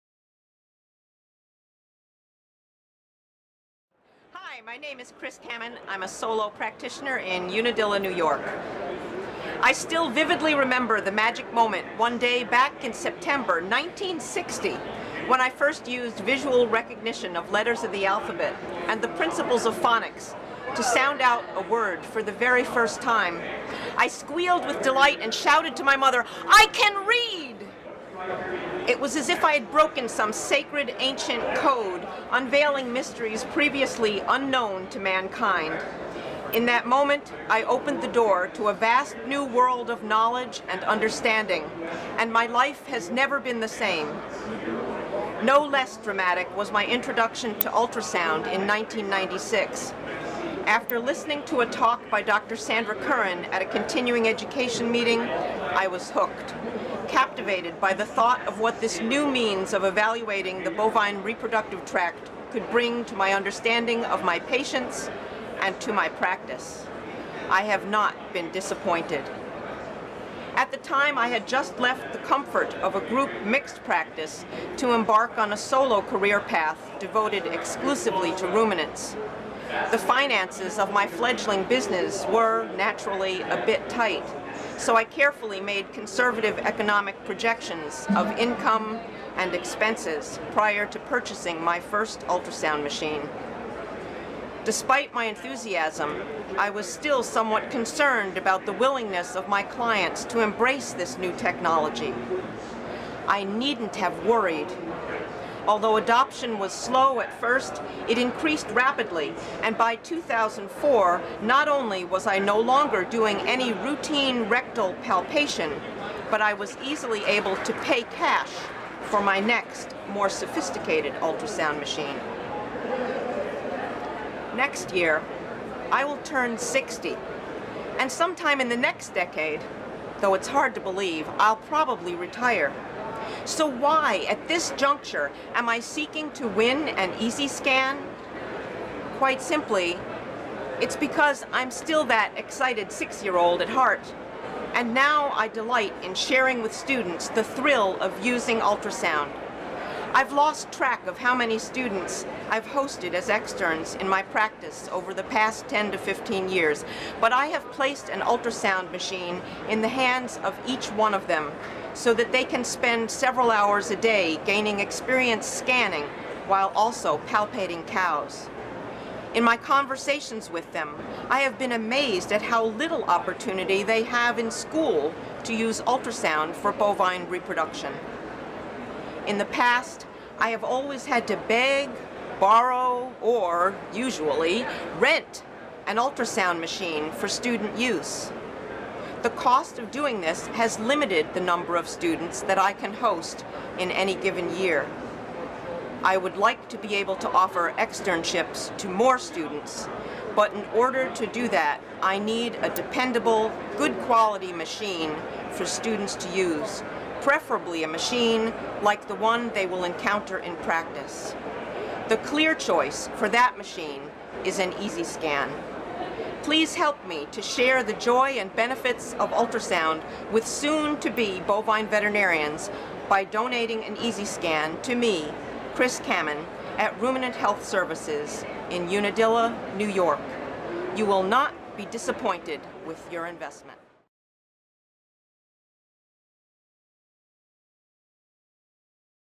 Dr. Christine Camann -- Shares passion of utilizing ultrasound with veterinary students
Dr. Camann connects how she felt when she learned to read to the doors opened by her introduction to ultrasound. She is still the excited 6 year old at heart and wants to share that passion with veterinary students about ultrasound through offering externships.

Dr. Camann is a finalist for the 2013 AABP Video Contest hosted by BCF Technology. Six favorites were chosen from 92 video entries to compete for the chance to win a complete Easi-Scan ultrasound system.

to vote for Dr. Christine Camann. Voting ends October 20, 2013.